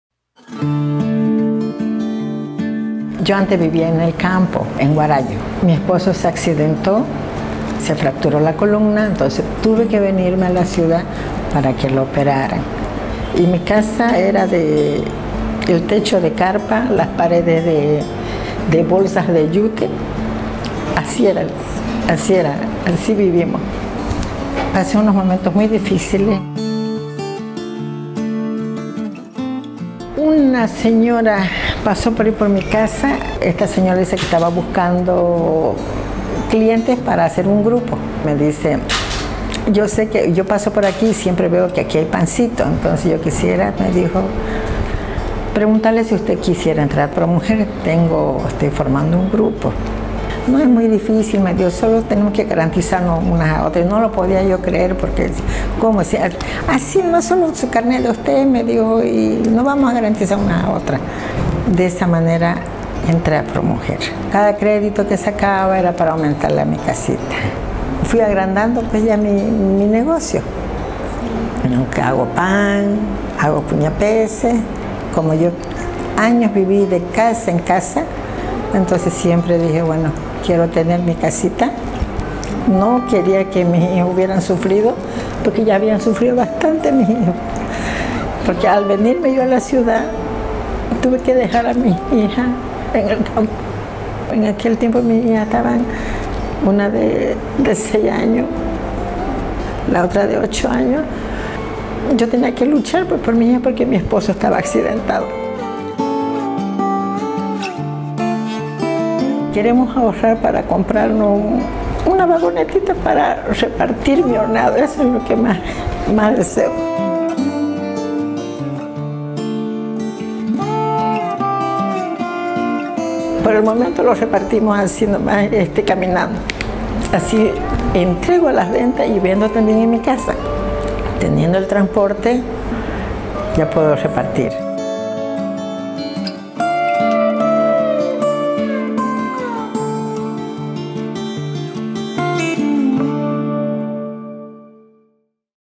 Testimonios Rosa Guzmán | Pro Mujer Bolivia
Rosa Guzmán, emigró hace muchos años de Guarayo a Santa Cruz en busca de mejores oportunidades. Vivía una vida precaria, aún recuerda el techo de carpa y las paredes de bolsas de Yute que tenía como hogar, además pasaba por una situación familiar difícil: su esposo se había fracturado la columna.
Rosa supo hacerle frente y triunfar. Junto a su familia ahora producen todo tipo de pan en su propia panadería, por eso, para ellos no hay días feriados, ni el Día del Trabajador, pero logran generar empleos y apoyar a sus familiares cuando lo necesitan. Ellos hacen parte de más del 60% del sector informal en Bolivia uno de los más altos, según estimaciones del Fondo Monetario Internacional.